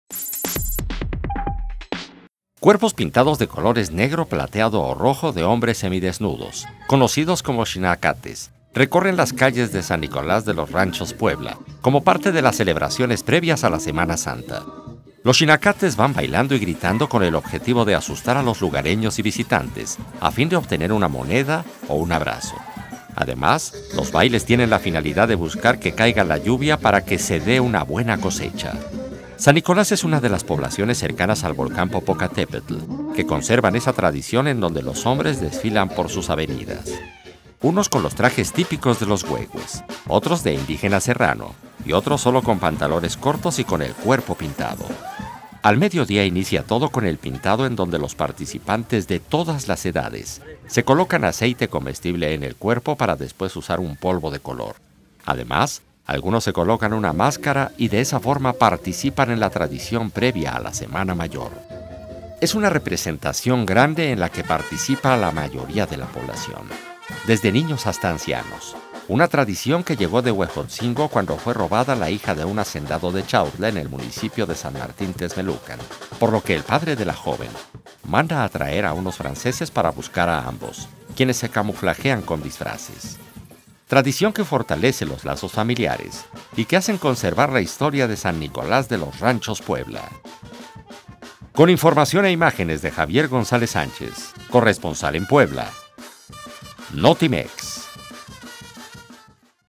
Celebran carnaval de los "pintados" en San Nicolás de los Ranchos, Puebla
Tradición llena de color, ritmo e historia en la que participa la mayor parte de la población